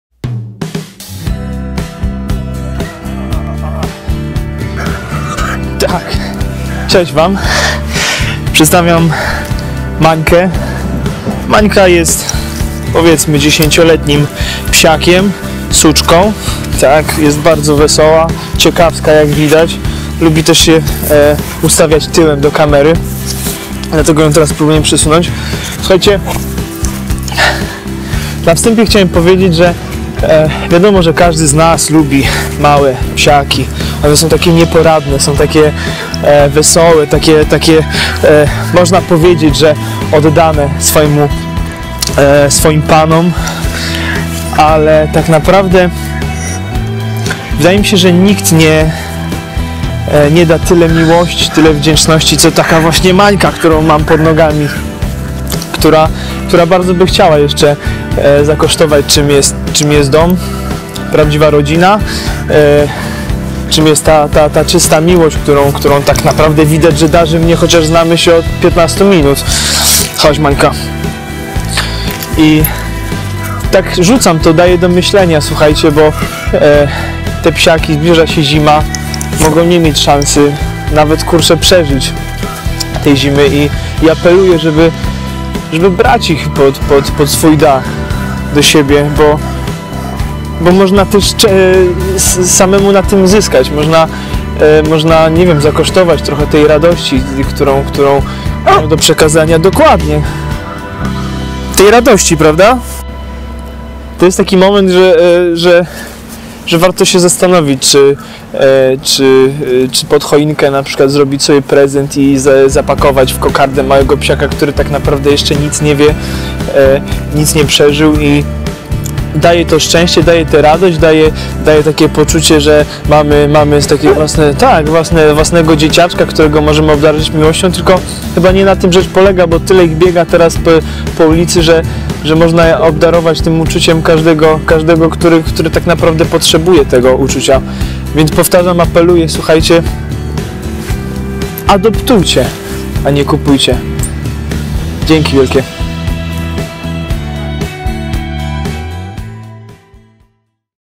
Pora na Seniora: Manka i Tomasz Włosok
Jeśli być seniorem, to właśnie takim jak Manka! Żwawym, radosnym i czasem tracącym głowę dla pasji! Czy Manka - schroniskowa weteranka zdąży poznać co to jest miłość, dom i kochający człowiek?

Zobaczcie jak Manka świetnie odnalazła się w towarzystwie aktora Tomasza Włosoka  i jak szybko mu zaufała!
Czy ta zima będzie dla Manki ostatnią? Z wiekiem coraz trudniej znosi się mróz, wilgoć, samotność. Manka chciałaby ostatnie lata spędzić w swoim własnym domu...

Manka jest w schronisku od tak zwanego "zawsze", czyli na pewno od 2012 roku, kiedy to fundacja Viva! przejęła schronisko...

To suczka średniej wielkości, zgrabna, z zawadiacko zawiniętym ogonem, o pięknym umaszczeniu. Kiedyś była nieśmiałym psem, jednak obecnie sama zabiega o kontakt z człowiekiem, do którego jest bardzo pozytywnie nastawiona! To chyba widać ;)

Manka zna podstawowe komendy i chętnie nauczy się nowych - smaczki, o które lubi się upominać przyjacielskim szczeknięciem, czynią cuda...

Manka jest suczką dość ruchliwą i lubi skakać, ale przywołana do porządku grzecznie siada. Uwielbia spacery, jednak nie muszą to być specjalnie długie wędrówki. Nie zwraca szczególnej uwagi na inne psy, mieszka w boksie z samcem.

Manka ma już na pewno 10 lat. Jest pozytywnie nastawiona do dzieci. Prawdopodobnie nie widzi już najlepiej, co może powodować, że wydaje się czasem ciut zdezorientowana i rozkojarzona... ale czy to nie urocze?

ADOPTUJ Mankę! Nie możesz adoptować? UDOSTĘPNIJ!